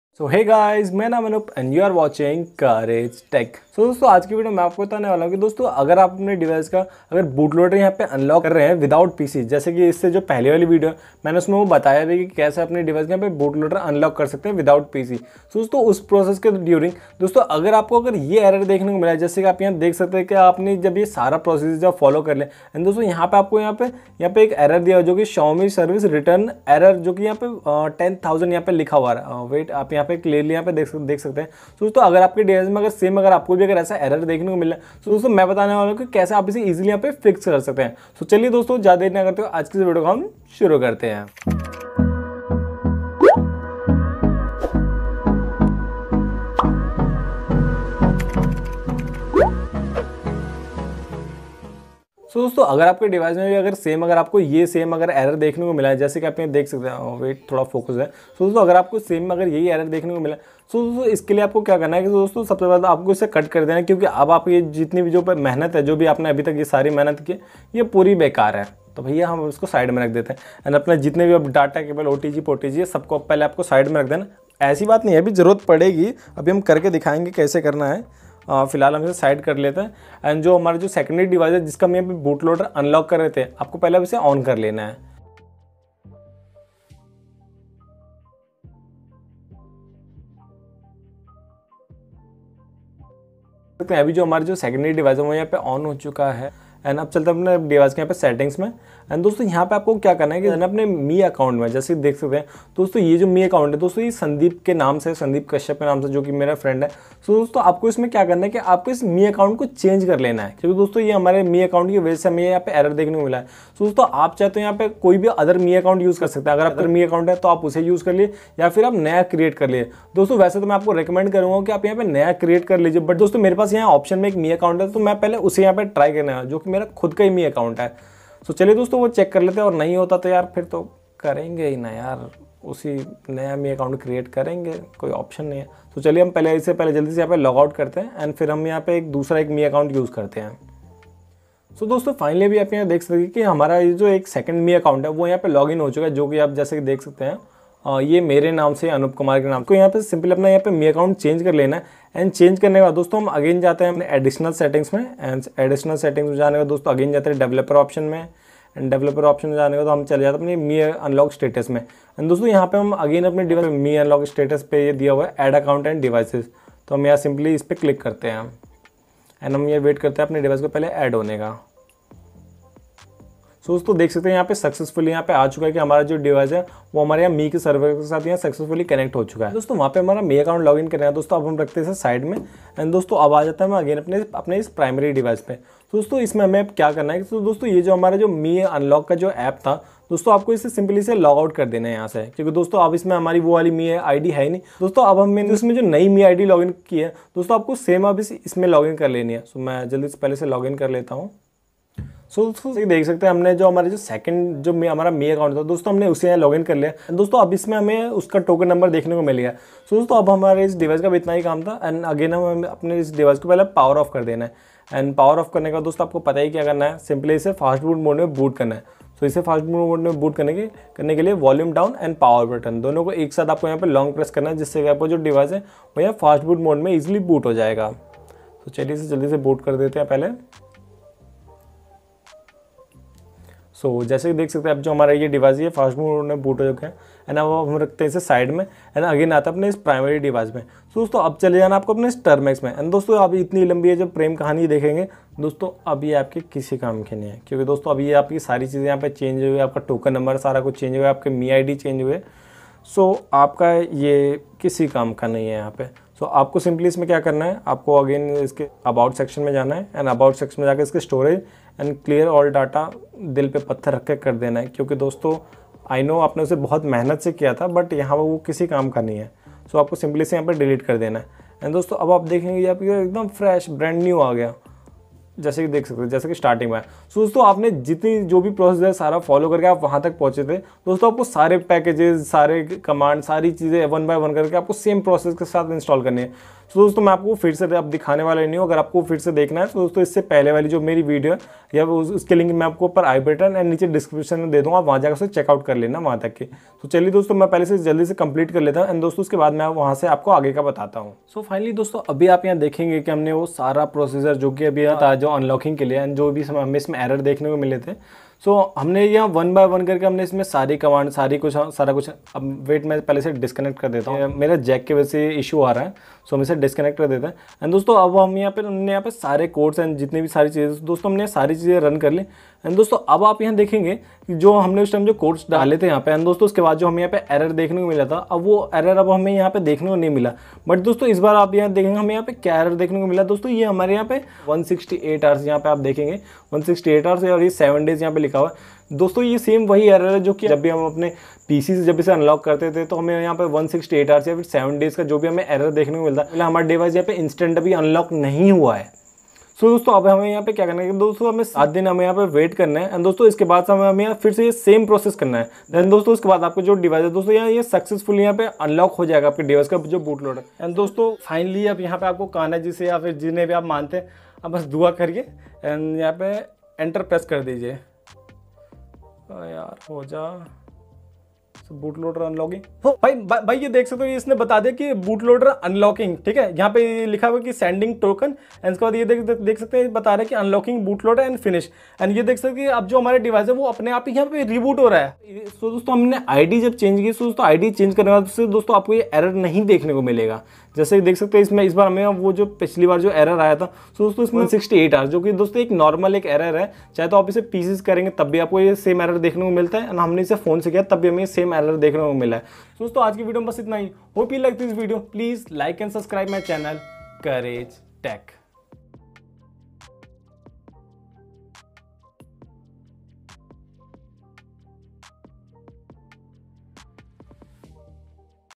Fix - Xiaomi service returned error 10000: while Unlocking xiaomi bootloader without PC
Fix: Xiaomi service returned error 10000: || Unlock xiaomi bootloader without PC error ||

Hello Guys mera naam hai Anoop and you are watching Courage Tech 😊😊😊

So guys is video me mai btaunga ki kese aap apne kisi bhi Xiaomi device ka bootloader unlock krte time
(Xiaomi service returned error 10000) Error ko fix kar sakte hai.

Note:- If you are unavailable to download files form my link.

[Direct link is available in telegram group]

🛑ALL LINKS

Courage Tech